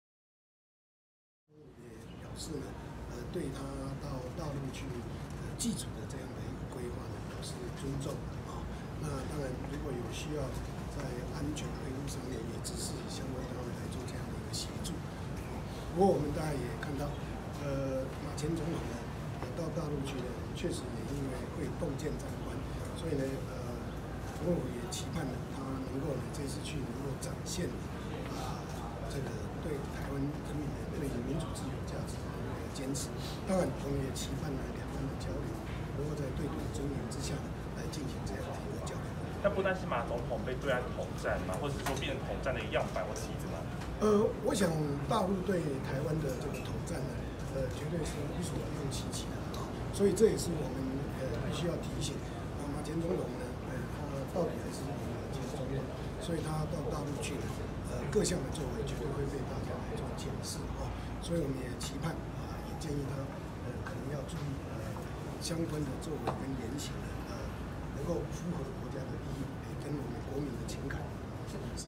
邱太三：馬英九赴陸動見觀瞻 盼兩岸對等尊嚴交流 | 中央社即時影音
前總統馬英九規劃將赴中國大陸祭祖，陸委會主委邱太三今天表示，馬英九到大陸去動見觀瞻，期盼馬英九此行能展現台灣人民對民主自由價值的堅持，並期盼兩岸在對等尊嚴下交流，不希望被矮化或統戰。（中央社記者王騰毅攝）